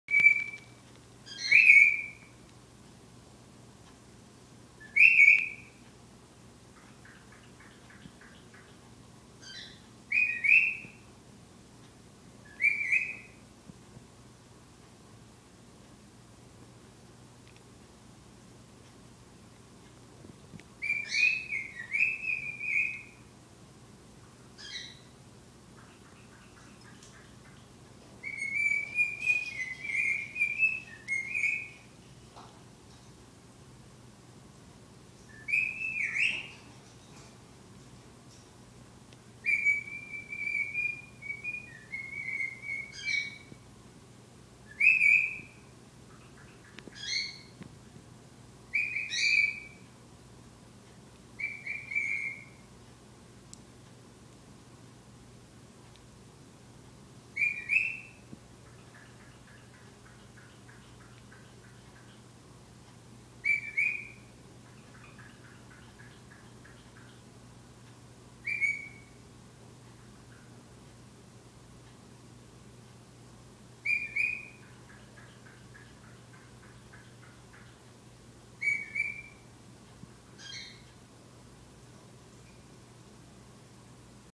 Cockatiels Singing!
My two cockatiel birds singing, couldn't show them in the video because they are shy and would stop singing.